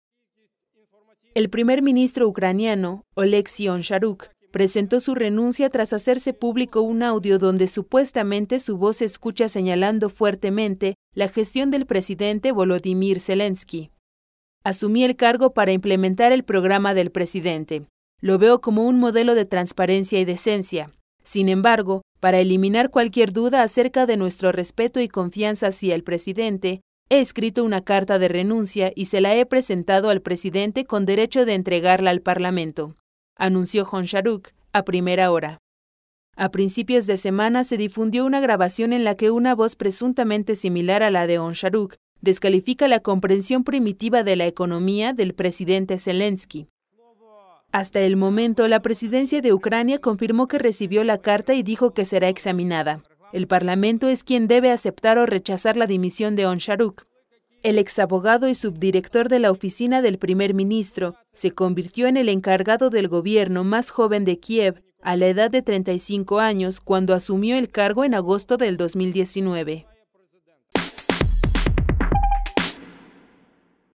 Renuncia el primer ministro de Ucrania
El primer ministro ucraniano, Oleksiy Honcharuk, presentó hoy su renuncia tras hacerse público un audio donde supuestamente su voz se escucha señalando fuertemente la gestión del presidente Volodímir Zelenski.